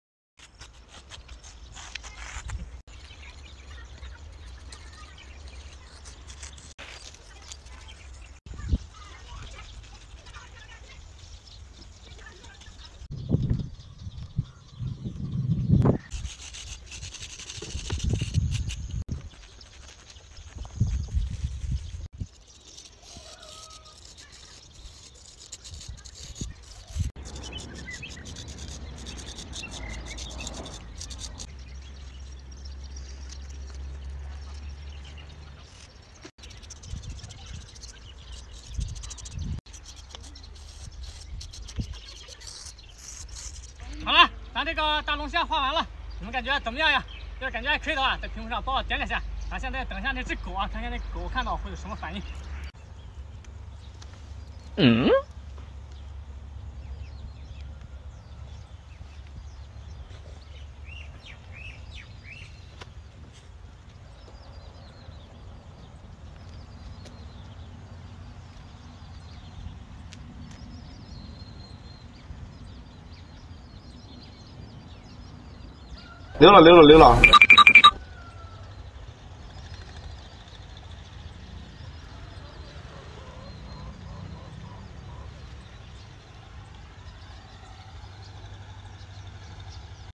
在街头画3D画｜3D Painting｜Street Graffiti｜Street Art【张世先】
Draw a  lobster  on the road, how will the dog ​​react?
在馬路上畫只龙虾，狗會有啥反應？3D立體畫｜街頭塗鴉｜街頭藝術｜